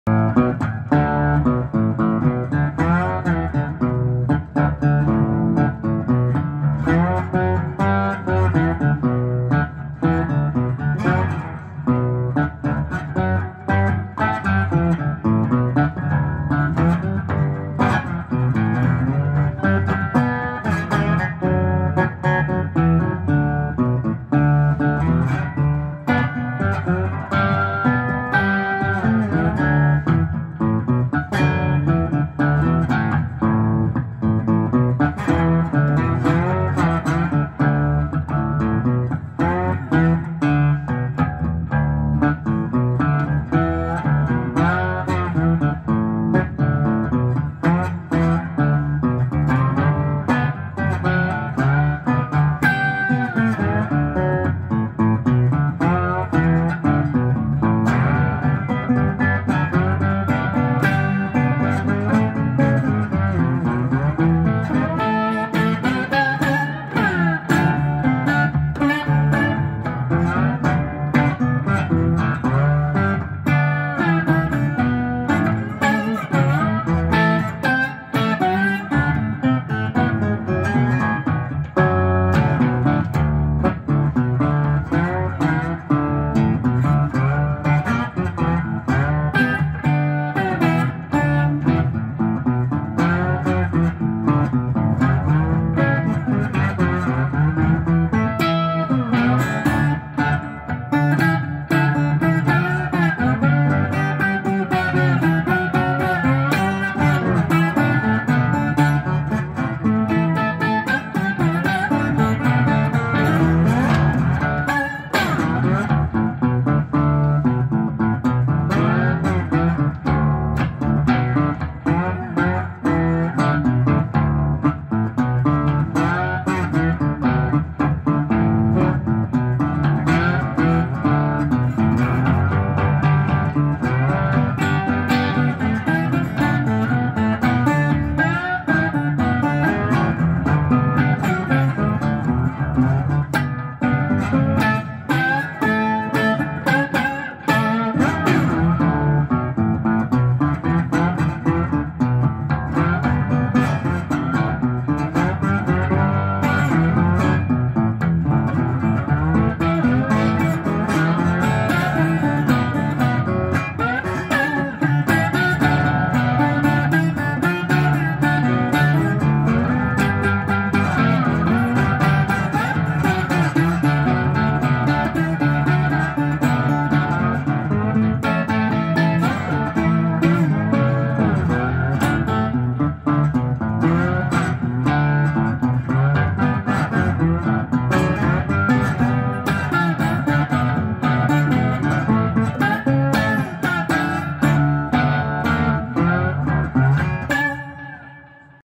noodling out a song maybe
noodling on the tricone maybe a song gets written, but trying variations on a theme.